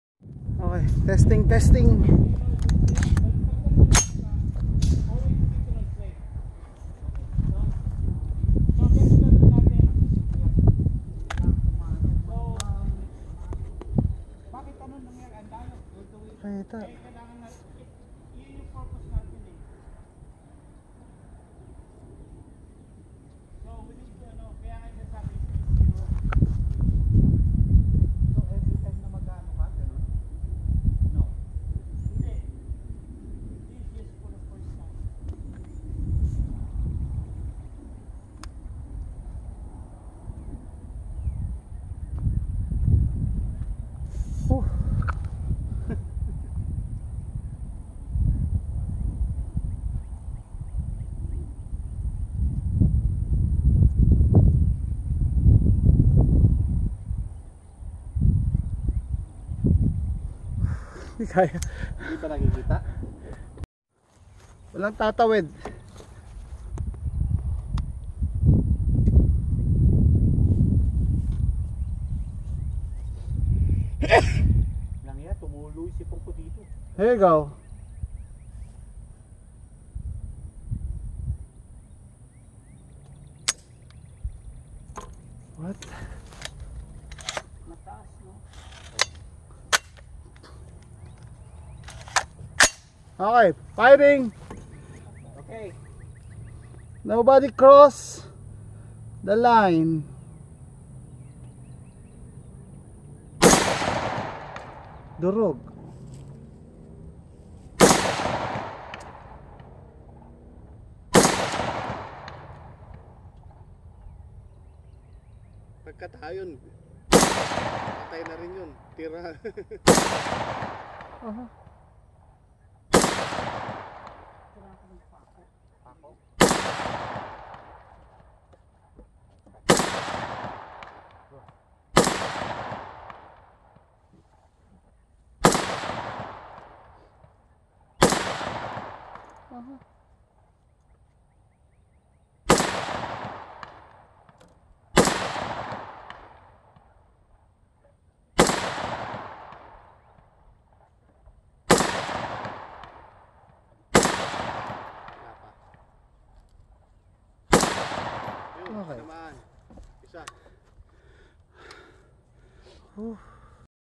TESTING NG AR KO SA FARM
LAST WEEK NAG FULL AUTO AKO, TESTING KUNG MAULIT KO YUN